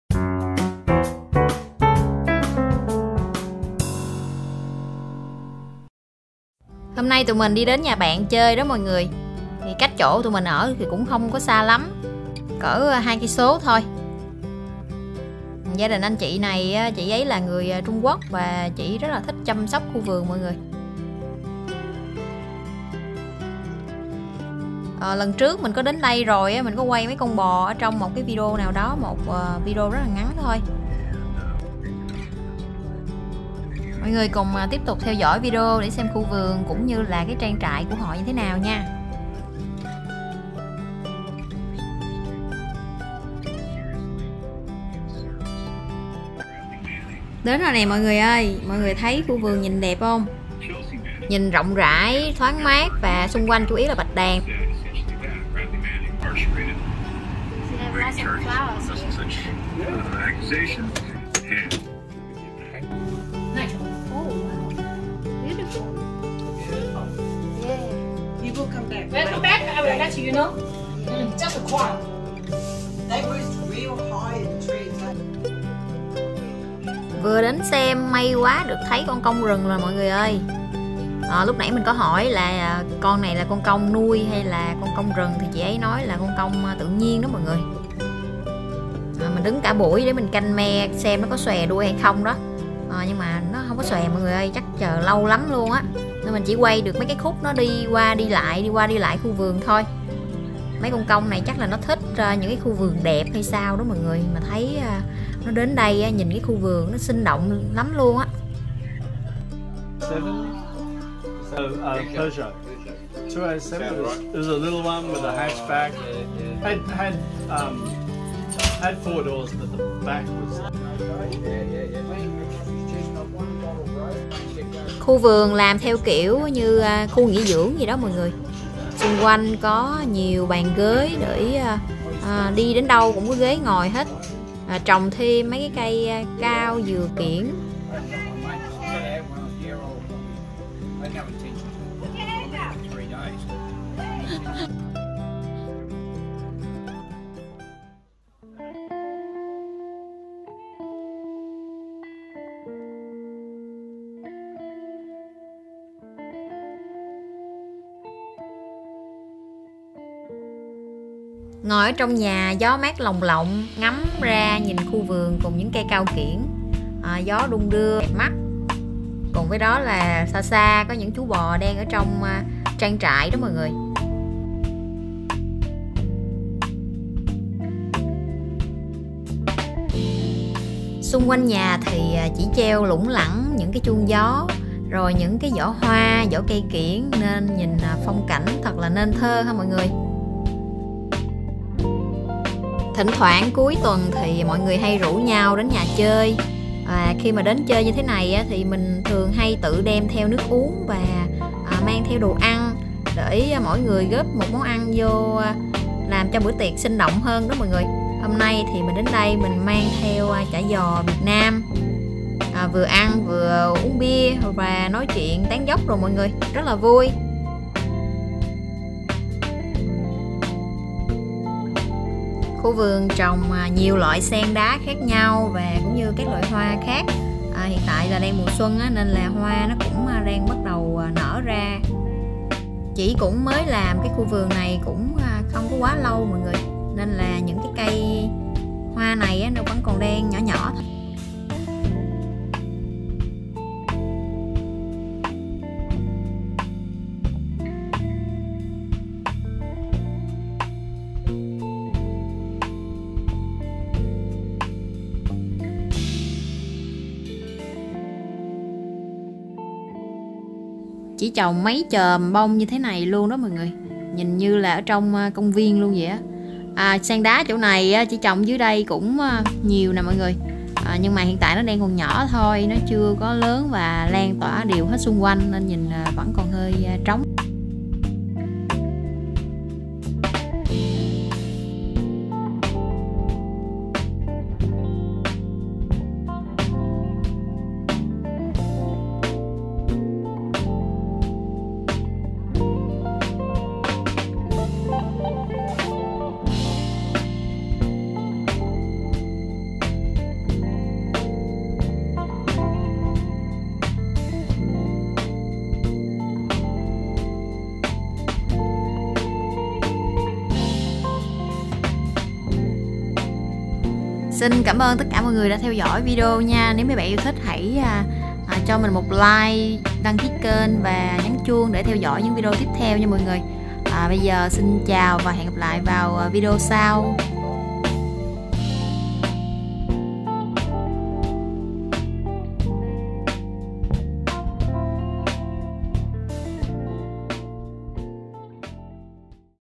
Tham quan TRANG TRẠI - KHU VƯỜN của người Úc | Chim công hoang dã đến vườn | CUOC SONG UC
Thăm trang trại nuôi bò nhỏ và khu vườn ở Úc bất ngờ gặp con chim công hoang dã đến vườn
👉 Một số video khác về nước Úc: